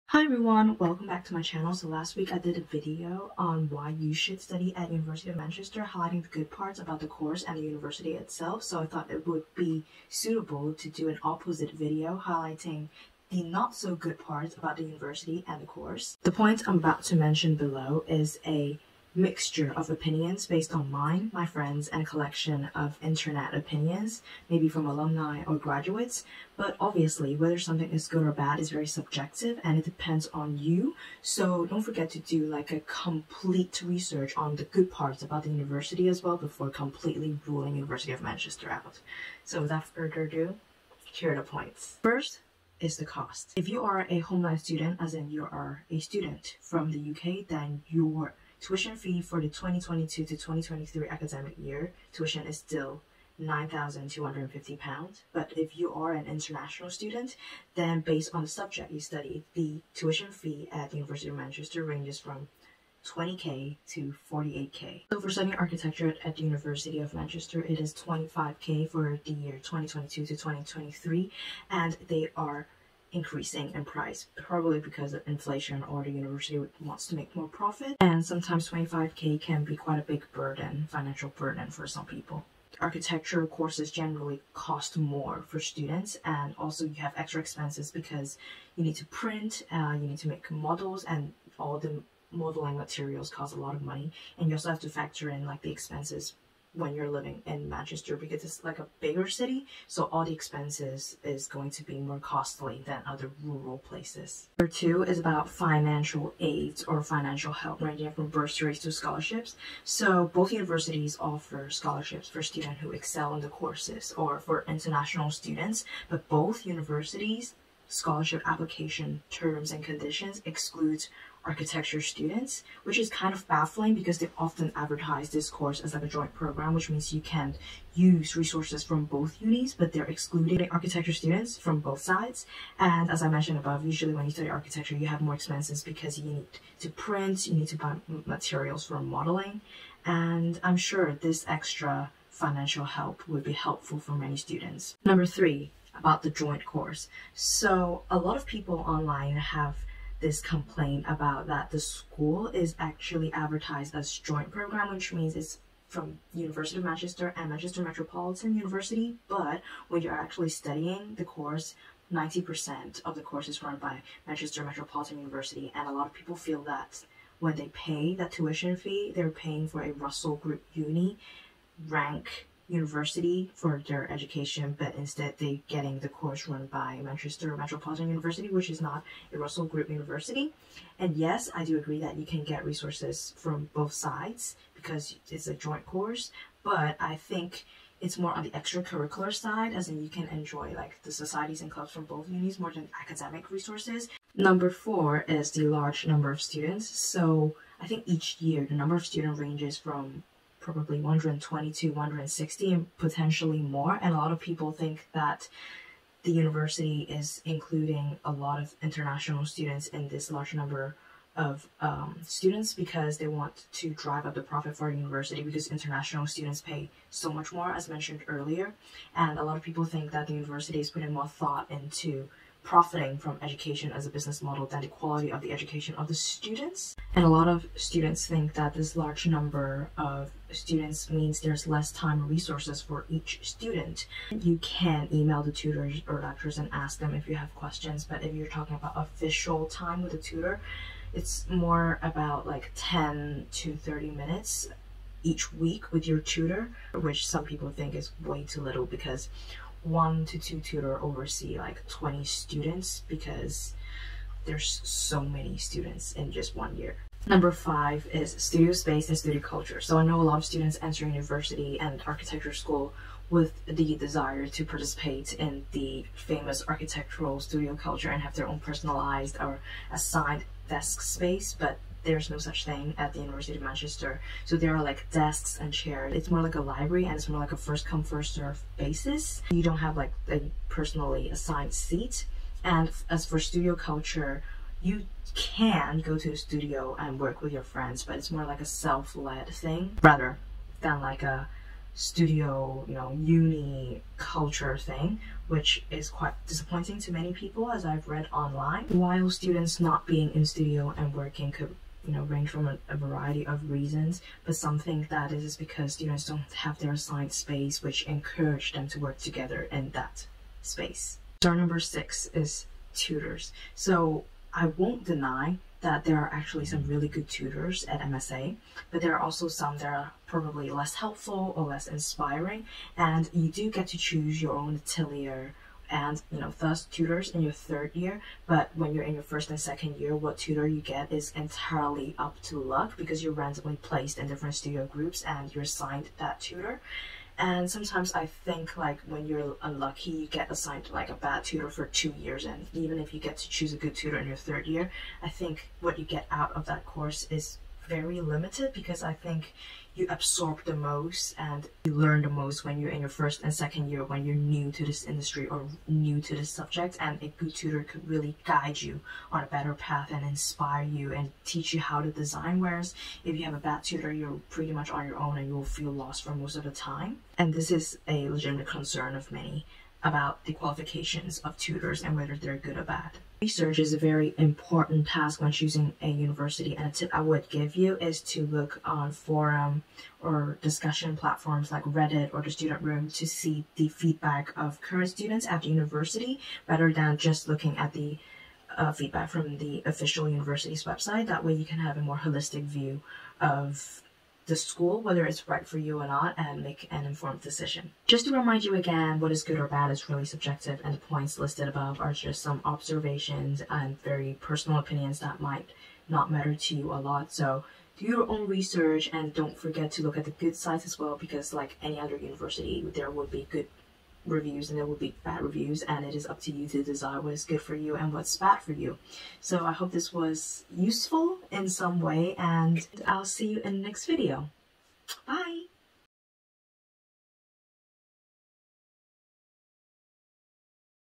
Should you study at the UNIVERSITY OF MANCHESTER?
In this video I will be talking about the slightly less appealing parts of the architecture course and the University of Manchester. These are just our opinions so what is good or bad is entirely up to you!

Follow me for more chaotic energy!

830